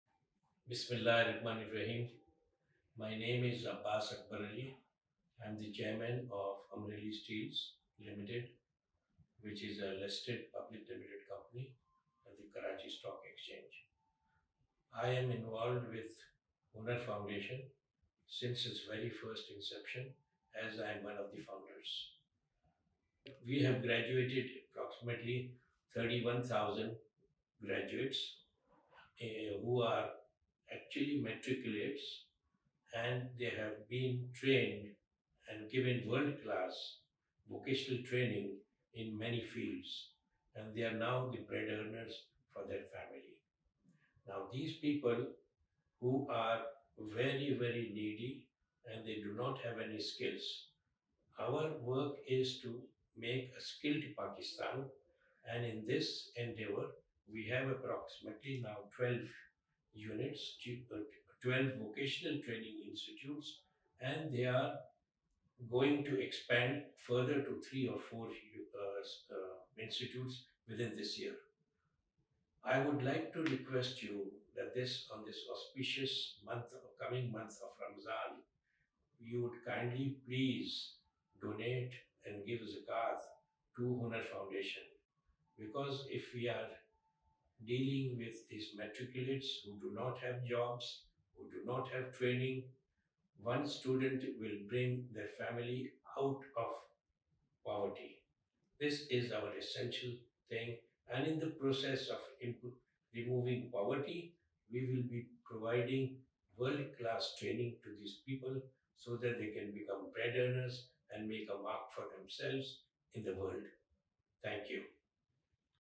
The Hunar Foundation | Mr. Abbas Akberali | Endorsement
Support The Hunar Foundation This Ramadan

Mr. Abbas Akberali, Chairman of Amreli Steels Limited and one of the founders of The Hunar Foundation, has been a steadfast supporter since its establishment. Recognizing the pivotal role of skill development in our nation's progress, he highlights the immense potential within our youth—a vital asset for shaping a prosperous future.
As The Hunar Foundation molds these young individuals into future leaders, Mr. Abbas Akberali encourages everyone to dedicate a portion of their Zakat this Ramadan to bolster The Hunar Foundation's mission.